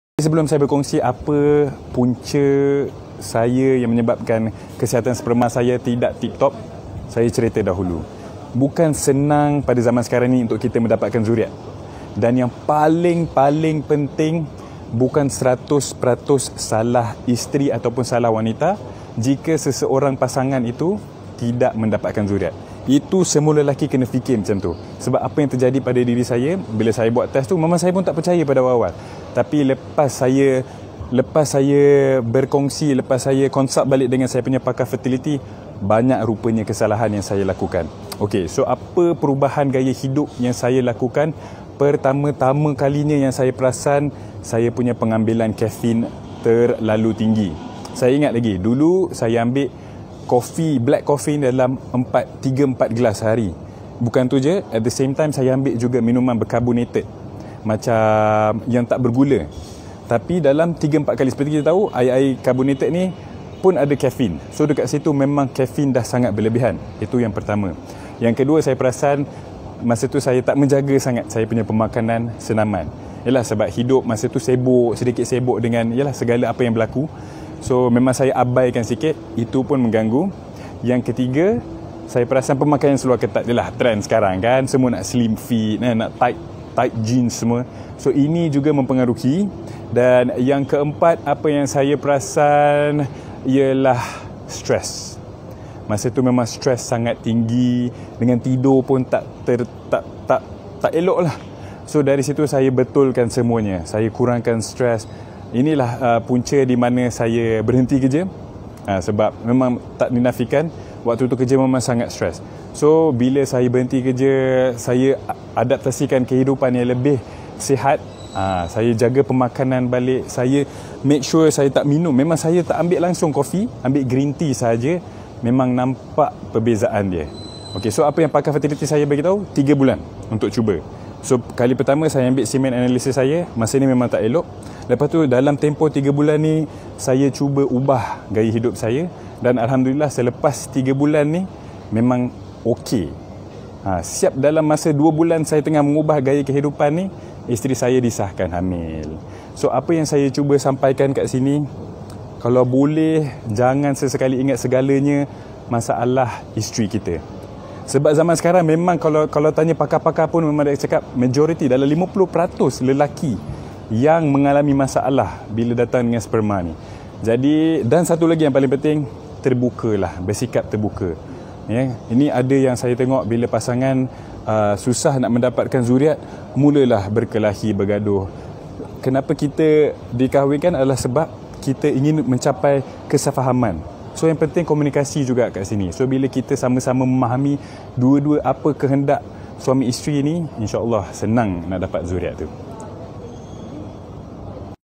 Pakai Seluar Ketat, Minum Kafein. Ganggu Kualiti Sperma
Bukan mudah untuk Dr say dan pasangannya untuk mendapatkan cahaya mata pertama mereka dahulu. Dr say sanggup mengubah gaya hidupnya apabila didapati kualiti spermanya terganggu. Ikuti sharing dari doktor ini tentang pengalamannya itu.